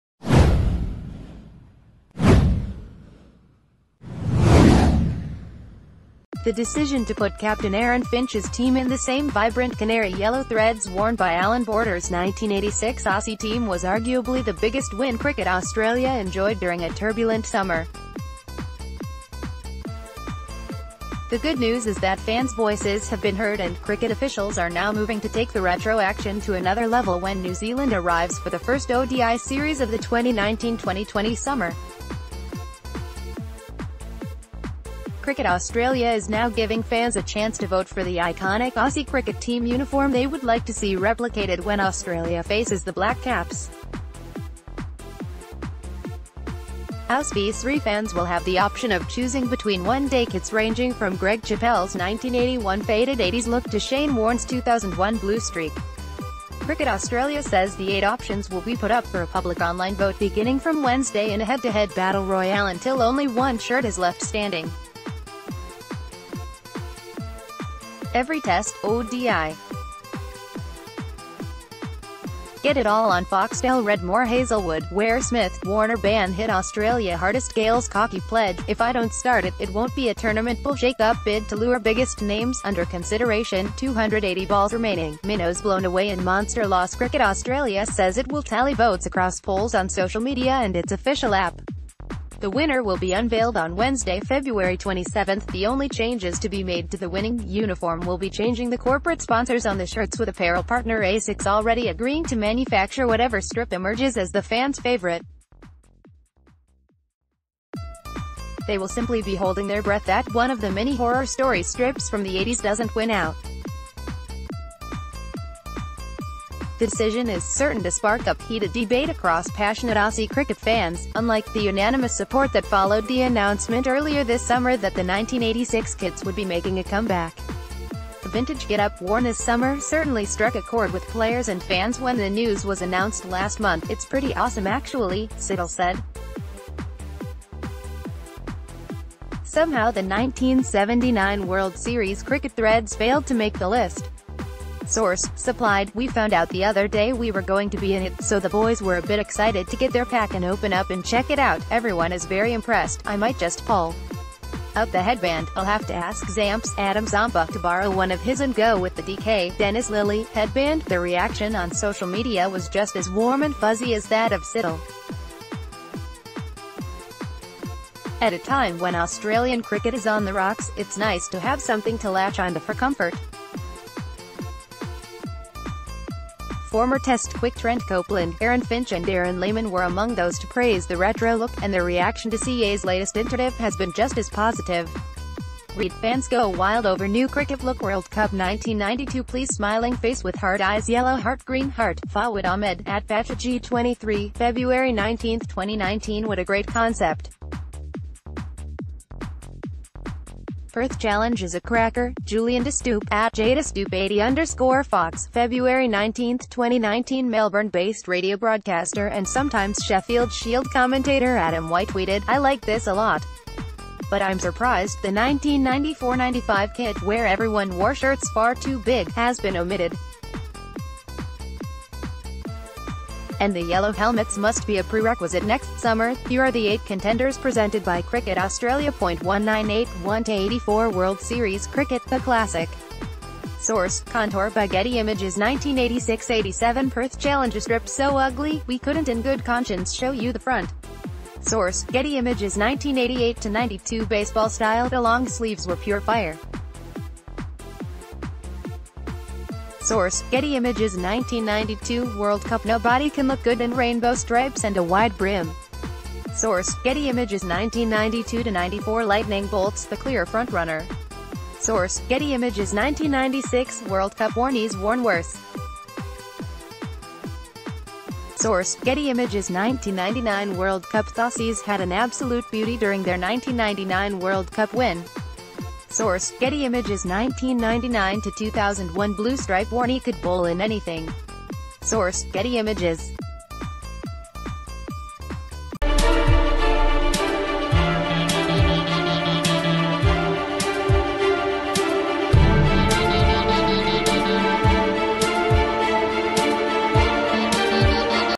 Cricket Australia delights fans with battle royale over best ODI uniform
The decision to put captain Aaron Finch’s team in the same vibrant canary yellow threads worn by Allan Border’s 1986 Aussie team was arguably the biggest win Cricket Australia enjoyed during a turbulent summer. The good news is that fans’ voices have been heard and cricket officials are now moving to take the retro action to another level when New Zealand arrives for the first ODI series of the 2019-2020 summer. Cricket Australia is now giving fans a chance to vote for the iconic Aussie cricket ...